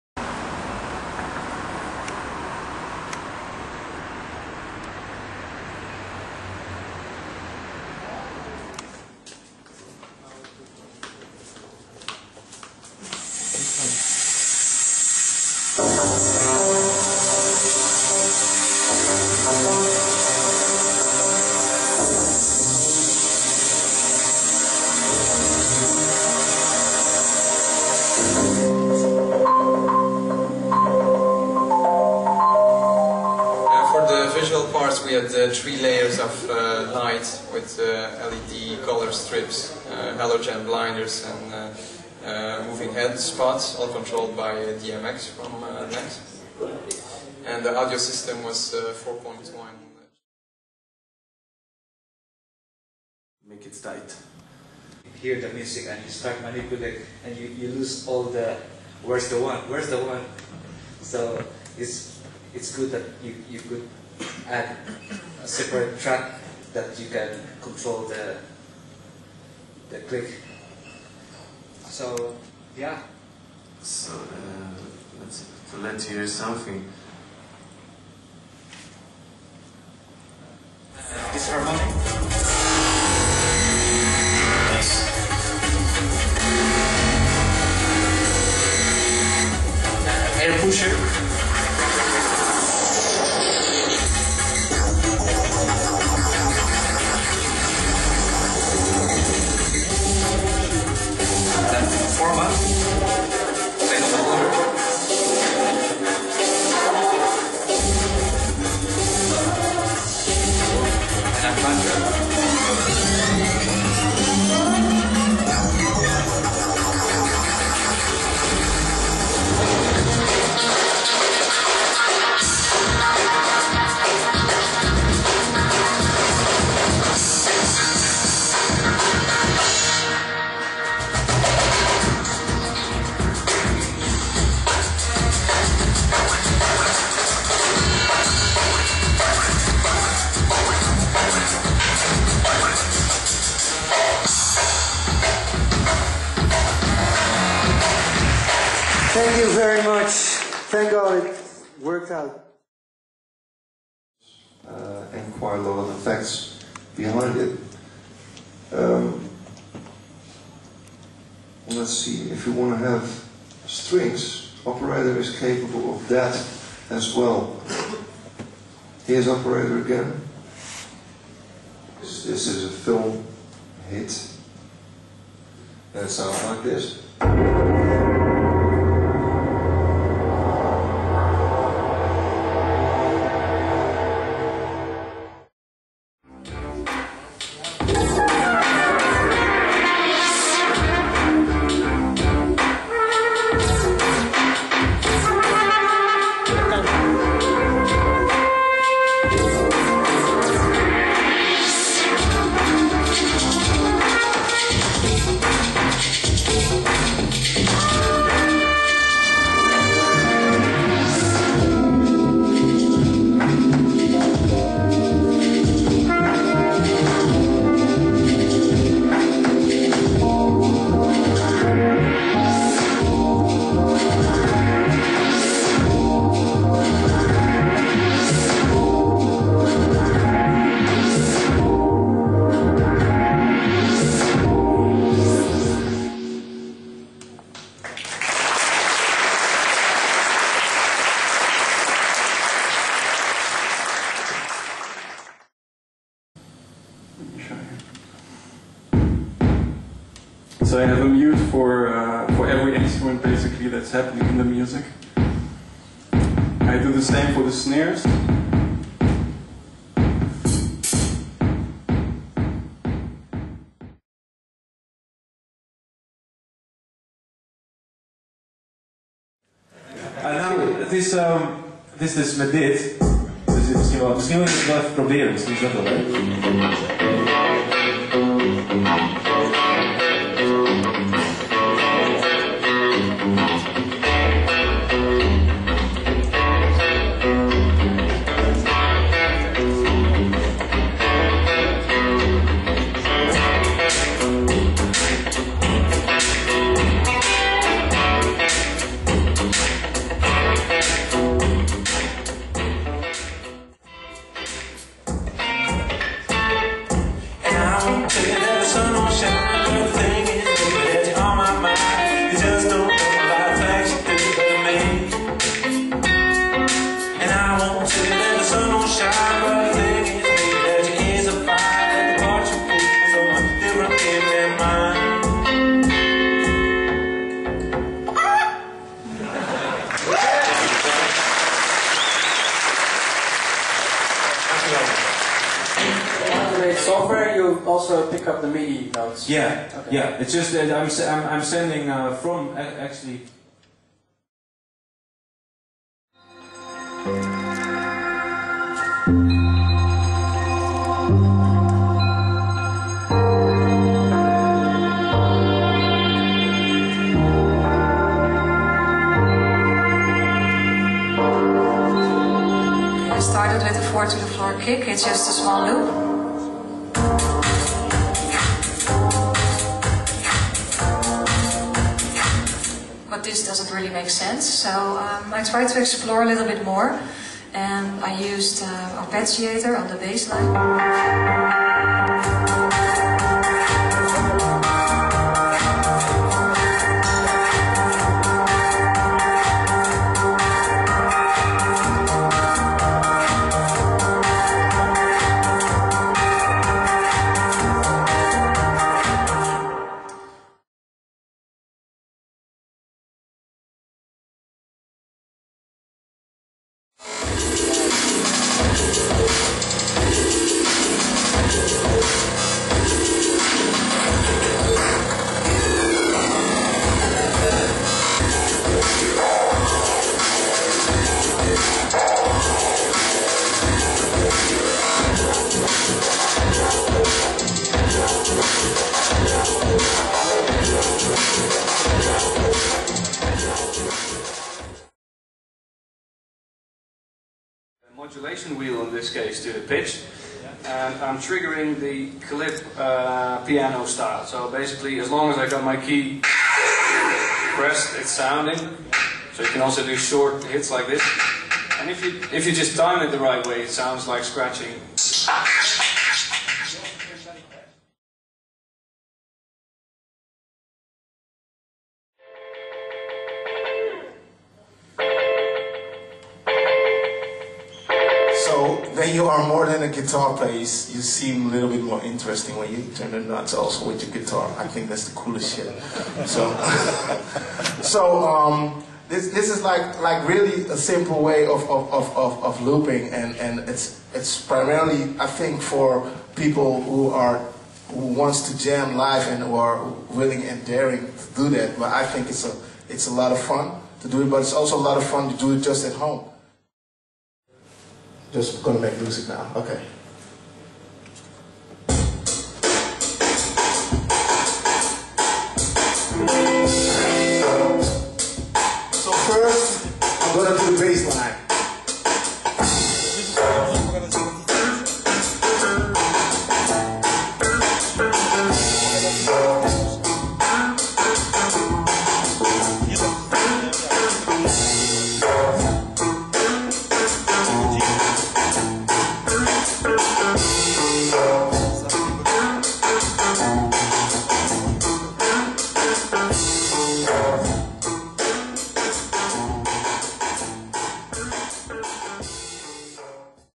Ableton VIP EVENT 2010 - all artists
Ableton VIP Event 2010 Rotterdam - june 12th 2010. Ableton professionals open up their Ableton projects to give a look inside their workflow and setup.
From guitar looping to VJ-ing, from joystick controllers to live drumprocessing..